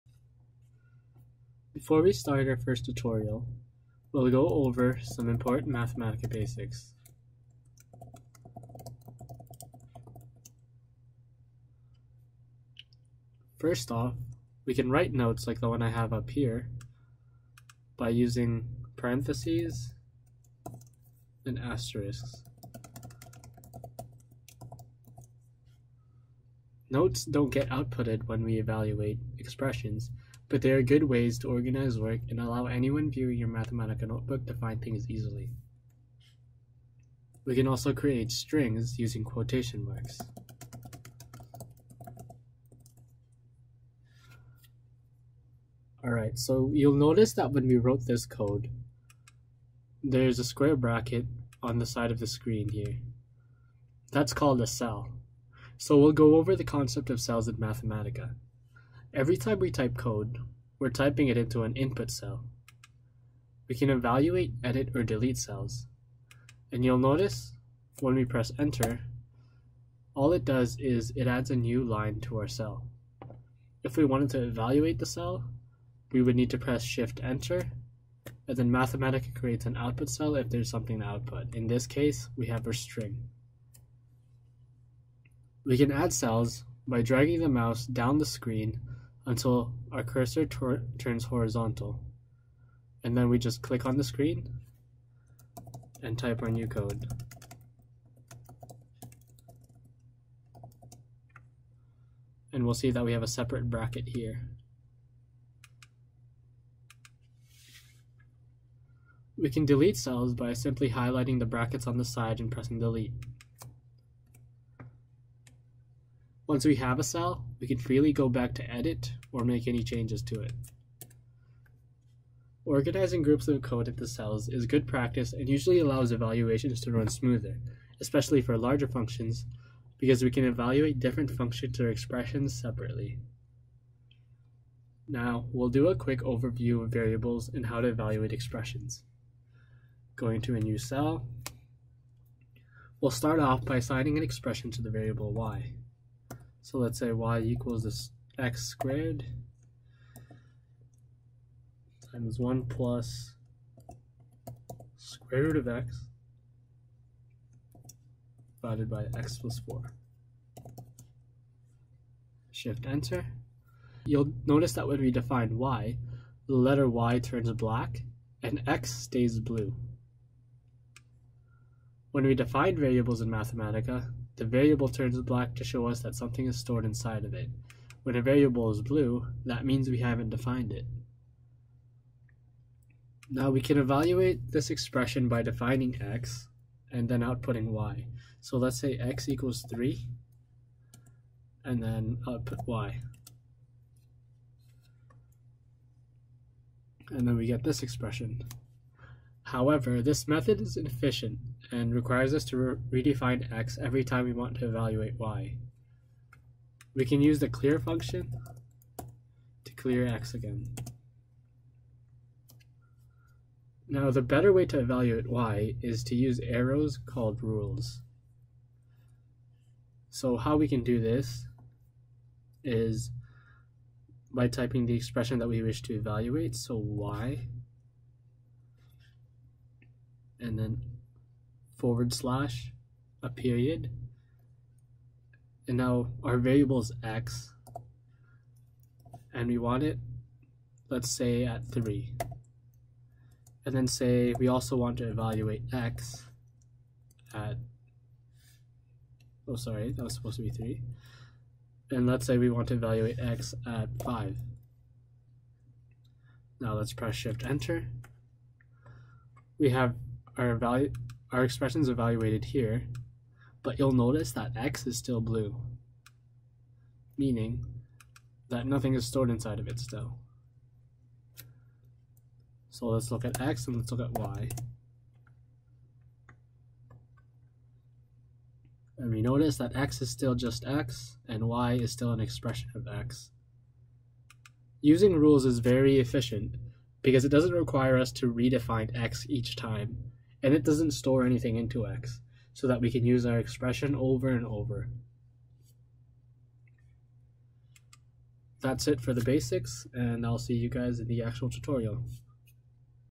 Lab 1: Part 0/4
This tutorial covers: Strings, Quotation marks, cells in Mathematica, variables, rules.

This tutorial was created by Arnold Vendiola and was supported by the Faculty of Engineering and the Centre for Teaching and Learning at the University of Alberta